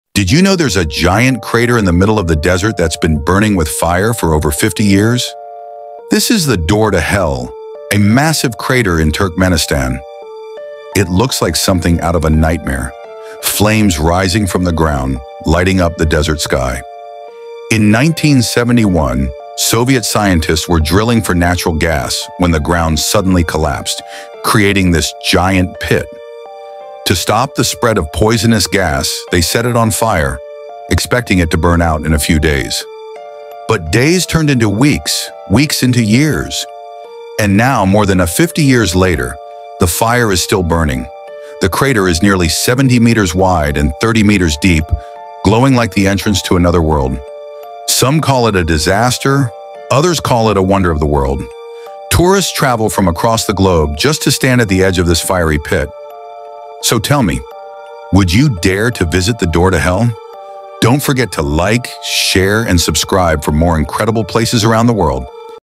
The Fiery Crater That Never Stops Burning
Did you know there’s a giant crater in Turkmenistan that has been burning with fire for over 50 years?

Known as the Door to Hell, this massive fiery pit was created in 1971 when Soviet scientists were drilling for natural gas. The ground collapsed, forming a huge crater. To prevent poisonous gas from spreading, they set it on fire — expecting it to burn for just a few days.

But more than half a century later, it’s still burning. The crater is nearly 70 meters wide and 30 meters deep, glowing like the entrance to another world.

👉 Would you dare to stand at the edge of the Door to Hell?